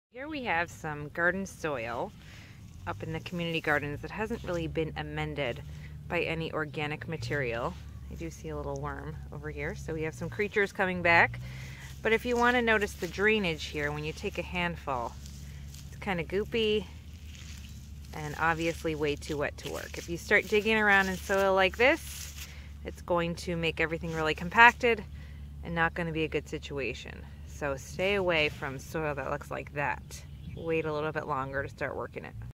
Too Wet Soil
Duke Farms demonstrates what too wet soil looks and feels like. Don't plant in too wet soil. Wait for it...